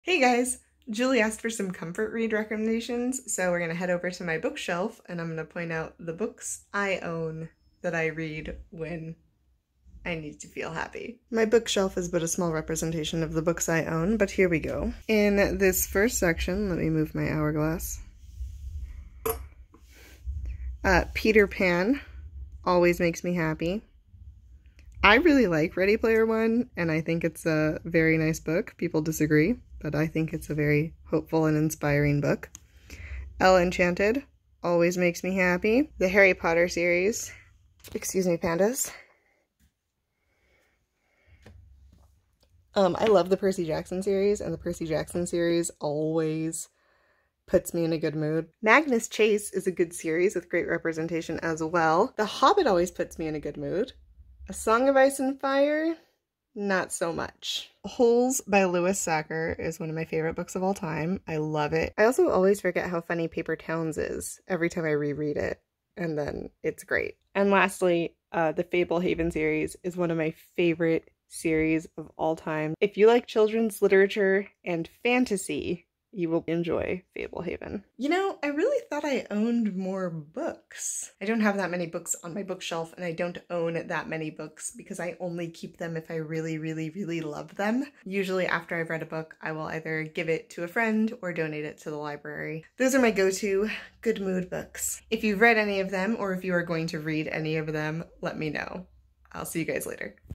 Comfort Reads | VEDA 18
Here are some of the books I always return to when I want to be in a good mood.